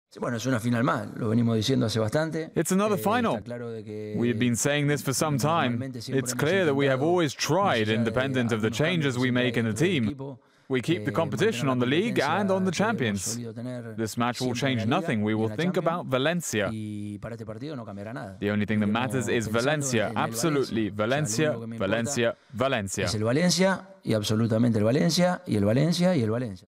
Diego Simeone only thinking about Valencia
Atletico Madrid manager Diego Simeone described Sunday's (27th April) Spanish La Liga game against Valencia as "another final" and said the death of of former Barcelona manager Tito Vilanova was a "tough loss".